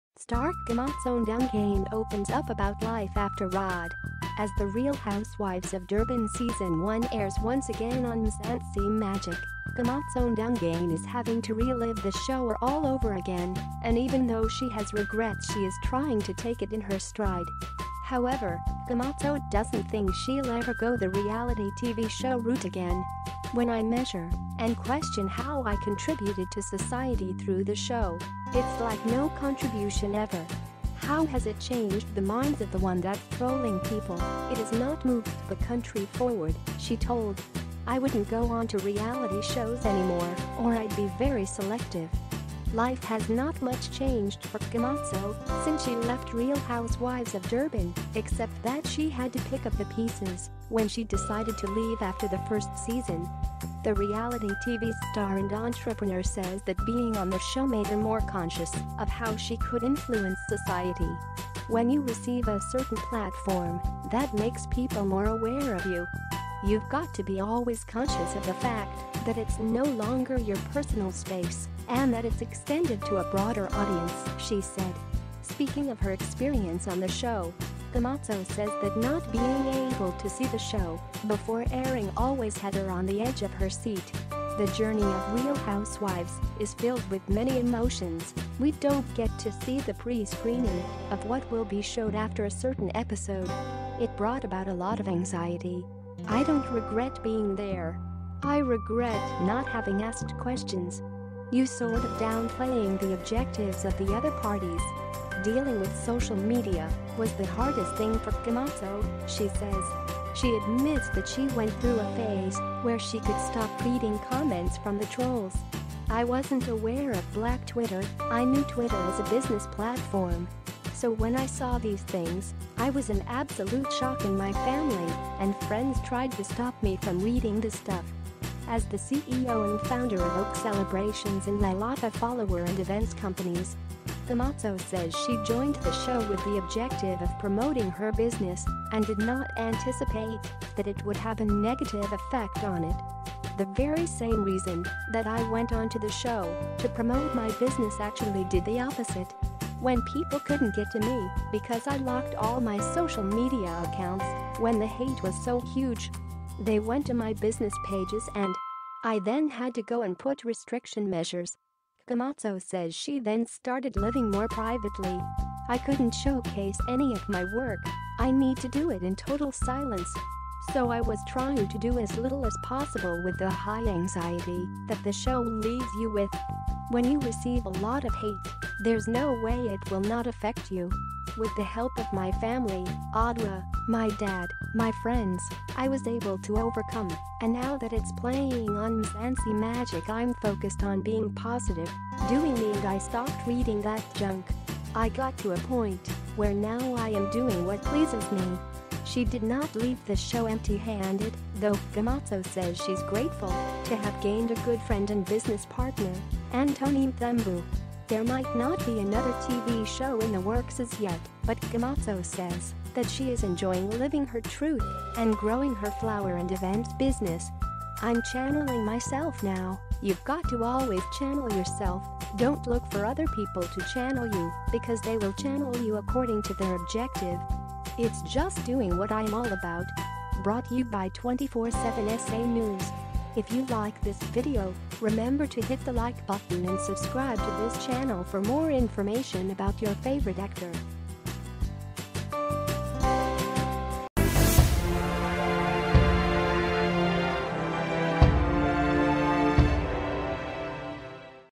star Kgomotso Ndungane opens up about life after 'RHOD'
As the Real Housewives of Durban season one airs once again on Mzansi Magic, Kgomotso Ndungane is having to relive the show all over again — and even though she has regrets she is trying to take it in her stride.